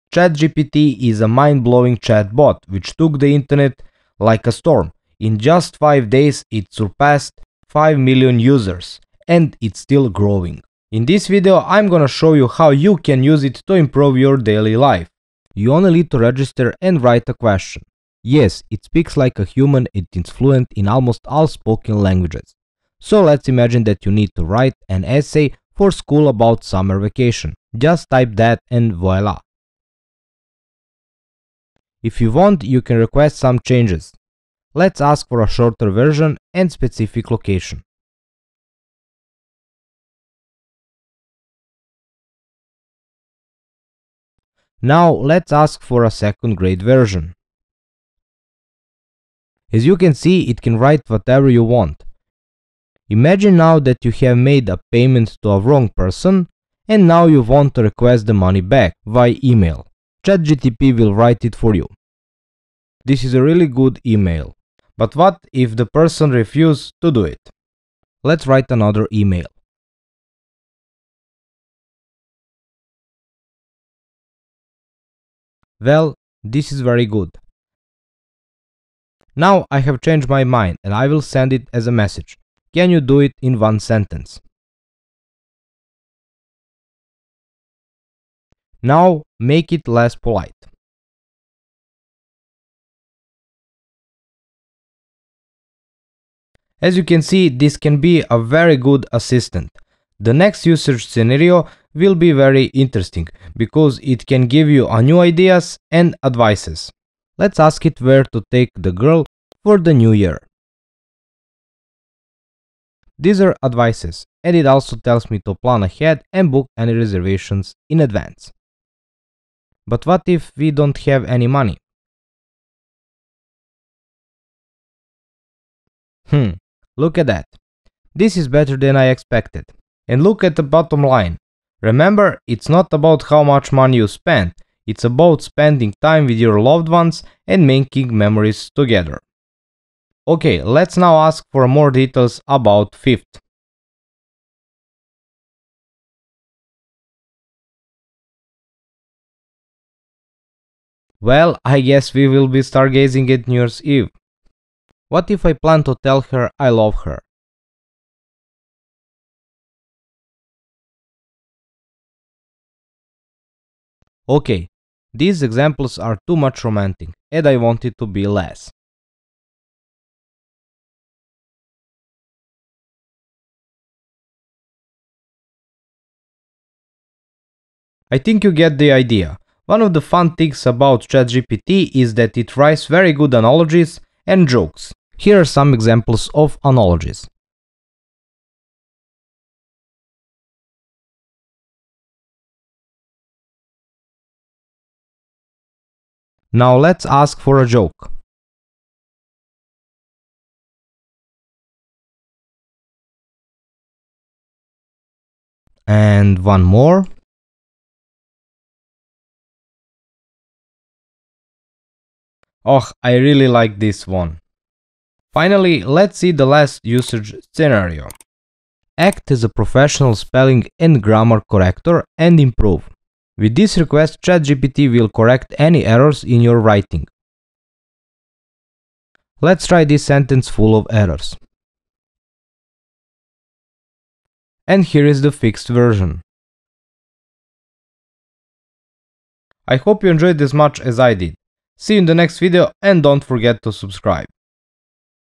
5 Great Ways to use Chat GPT
5 Ways to use Chat GPT secrets that will improve your life and how to use it. These are very useful secret tips that you can try right now. Chat GPT is an artificial intelligence tool that can generate text, write essays, answer questions, classify and compare text, explain or write codes. It is fluent in almost every language. This chat bot is most advanced AI to date and it's made by Open AI.

CHAPTERS
00:00 Intro
00:18 Writing story or any other text
00:52 Writing e-mail or text message
01:44 Ask for ideas and advices
02:57 Chat GPT and analogies
03:34 Chat GPT correct grammar